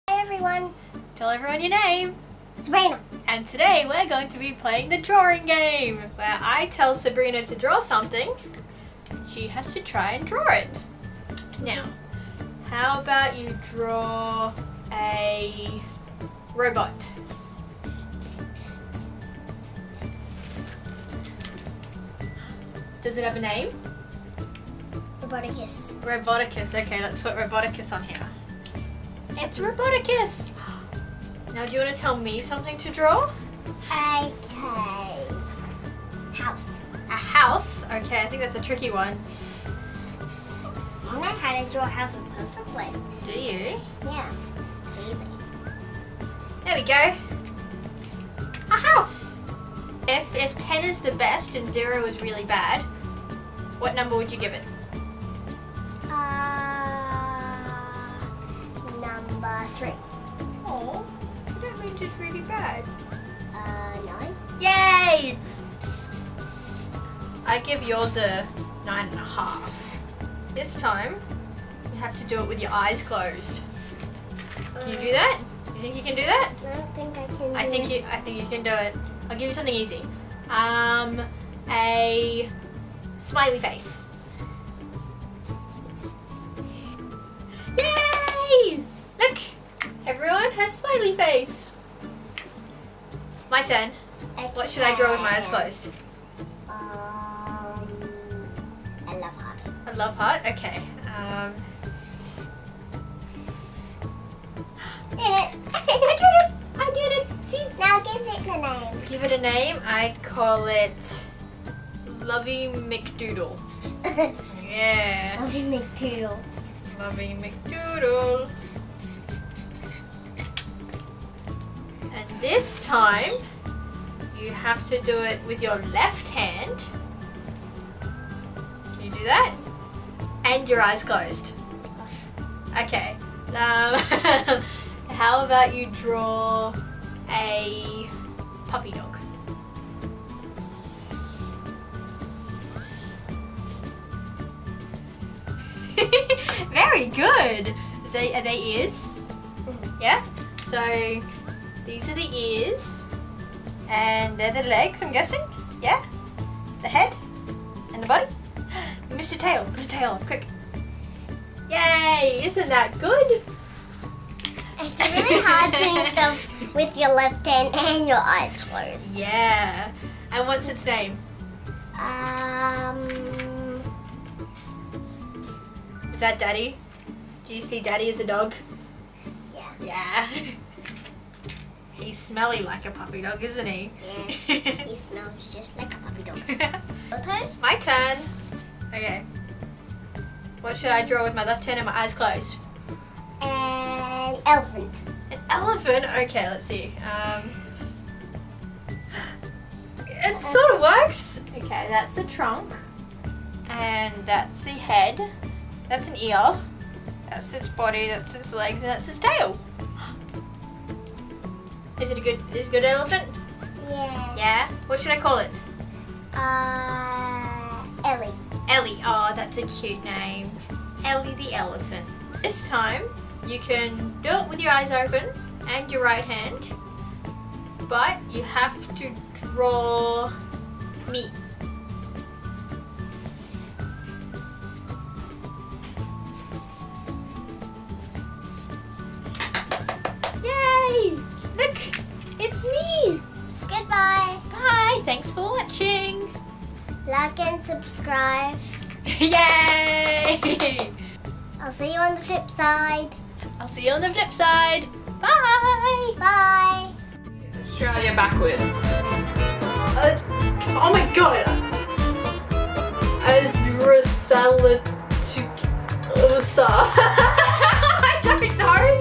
Drawing with my Niece!
Meet my 6 year old niece Sabrina! I filmed a few videos with her and now all she wants to do is make movies! *so proud*
We did the drawing game, but a bit more child friendly, and then we went on and made a puppet show from my Harry Potter puppets so stick round if you wanna see that! It's coming soon :)

Get in touch with me on other social media like...